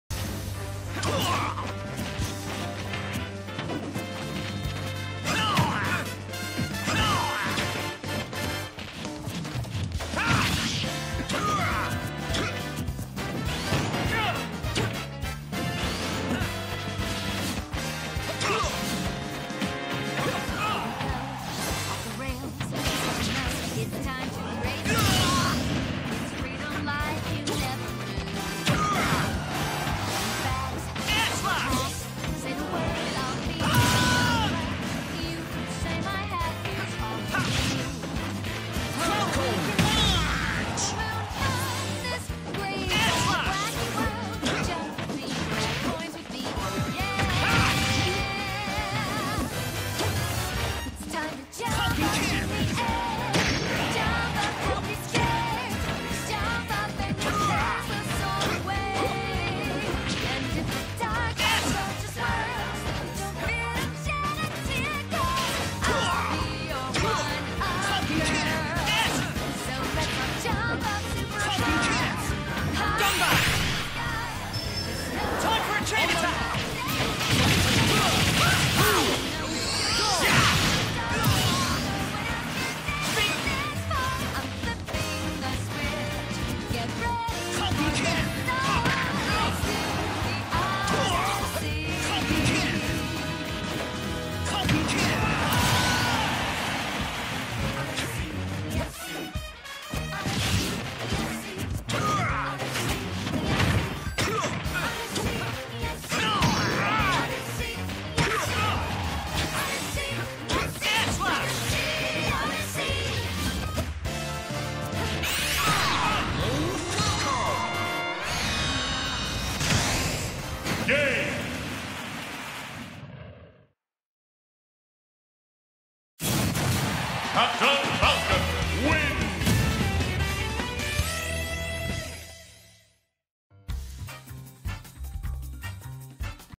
ReynTime's Last Stand (Amiibo Fest Tourney 16)
At this point all Amiibo Fighters have been wiped out of the tournament thanks to a lot of spirited CPU Players. Except for one which ReynTime has one final chance to get in the Semifinals. If he can get passed the powerful Blood Falcon first.